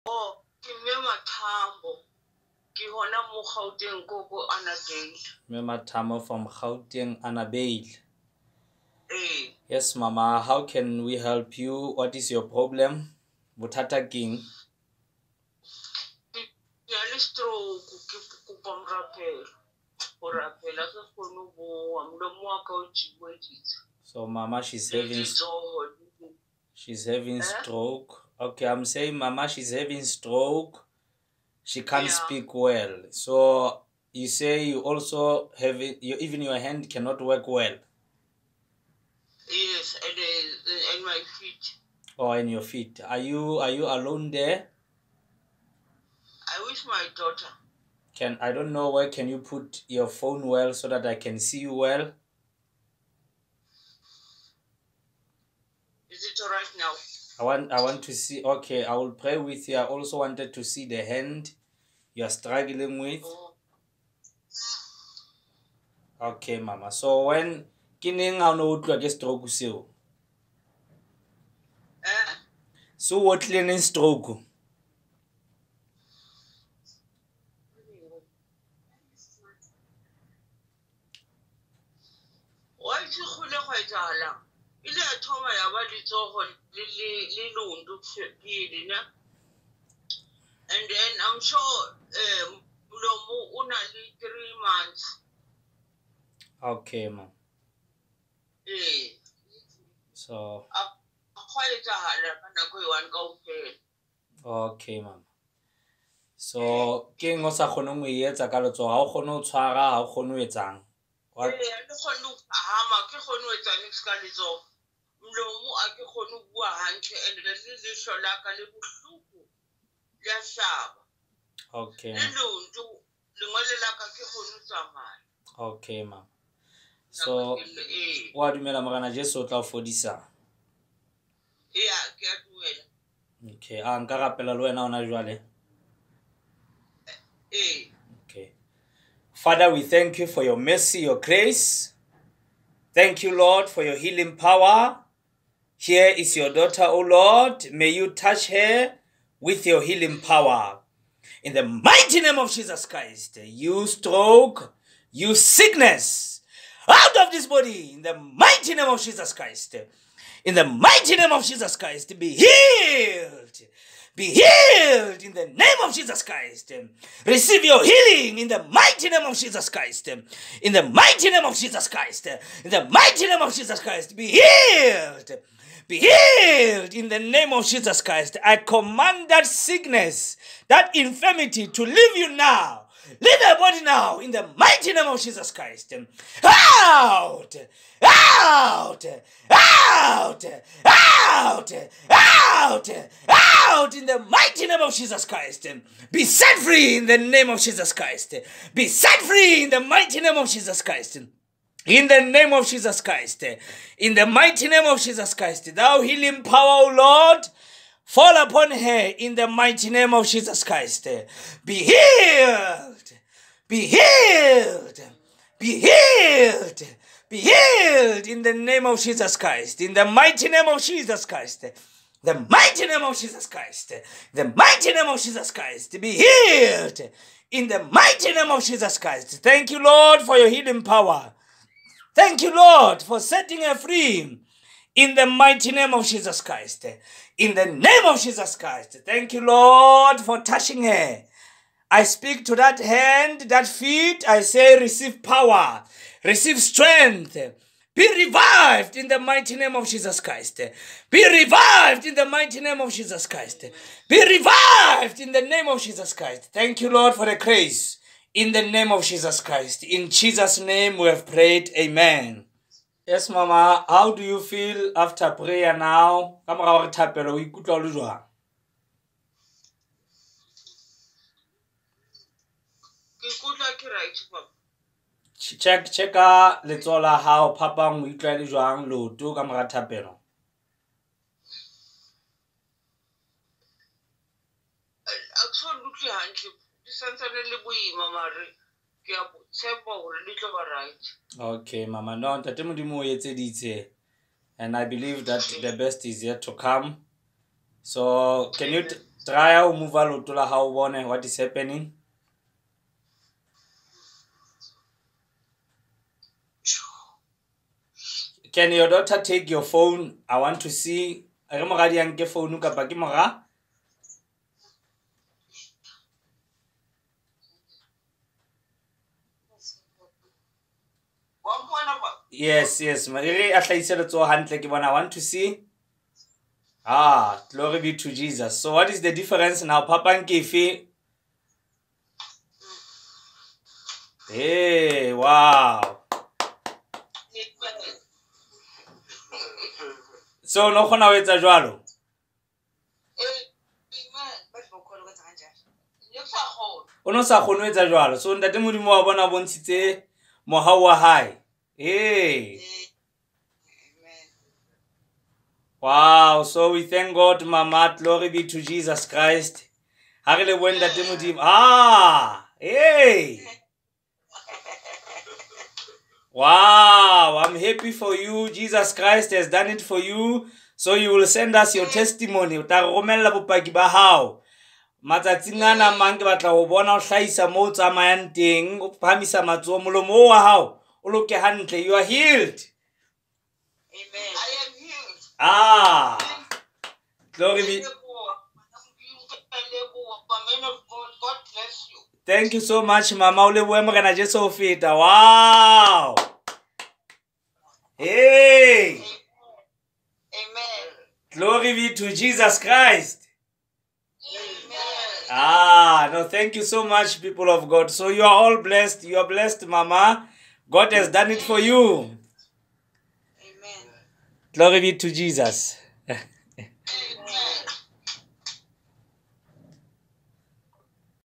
HEALING FROM STROKE||WATSAPP PRAYERLINE
Watch a 70 years old woman as she celebrates her healing from stroke.

Prayer requests
"Good morning  man of God  I'm from  gauteng ennedale I've got a  stroke on my right hand and foot, also my mouth I cannot speak well, I'm asking for  prayers please man of God help me."

Watch after prayer the Hand of God.